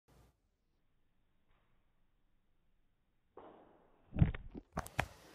Slo-mo Stair Flight 3
Slow motion pet southern flying squirrel (assapan) flying down stairs and head-butting a camera. The squirrel was not hurt, but the camera was completely destroyed.